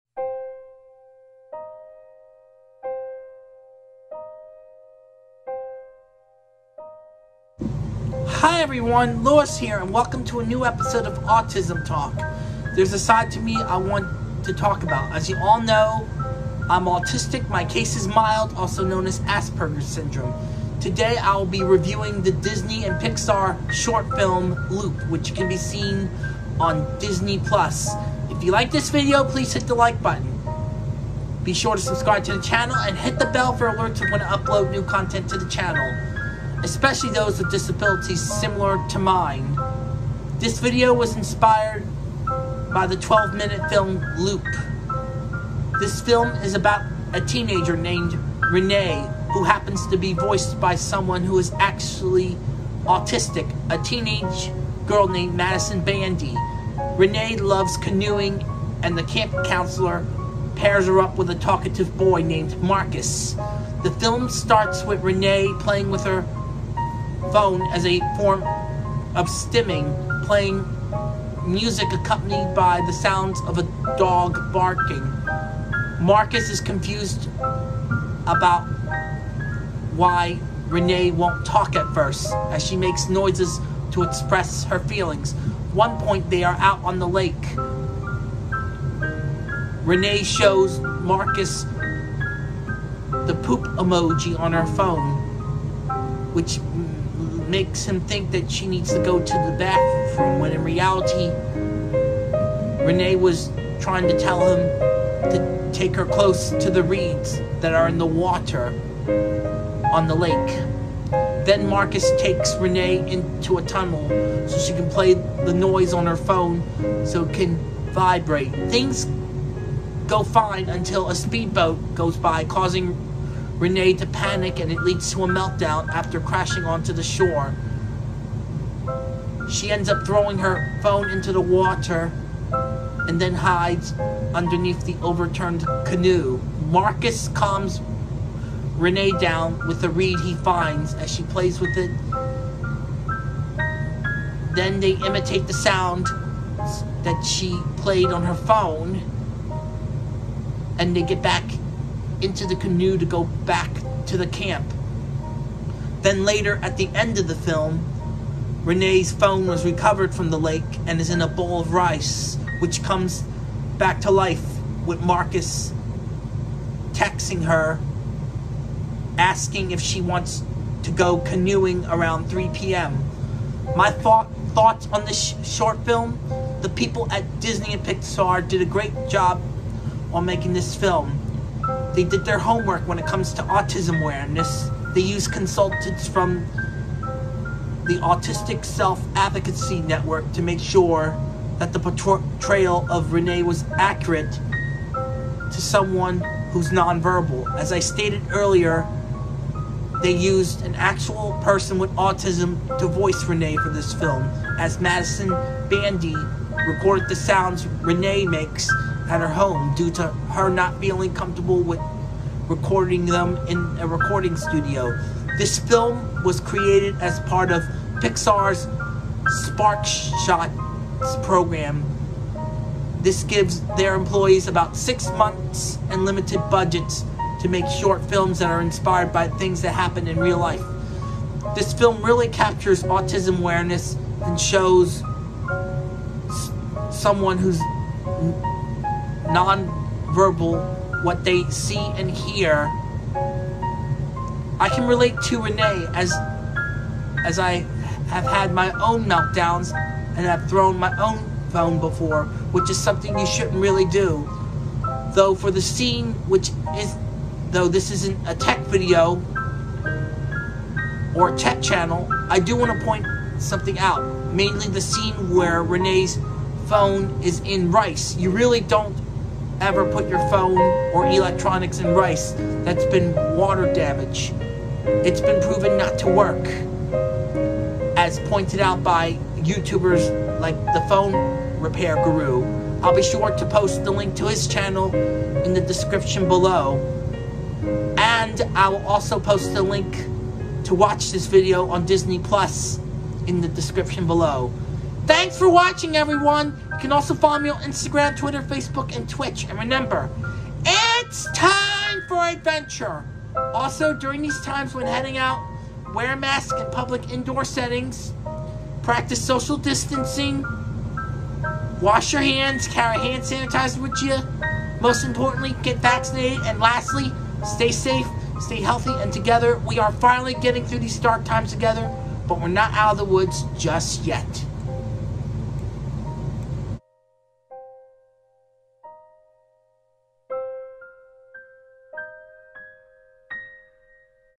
Autism Talk Episode 27 (Loop Review)
In this video I talk about & review the Disney/Pixar movie Loop.

PhoneRepairGuru

Loop on Disney+

Track

My Mersona

Diveheart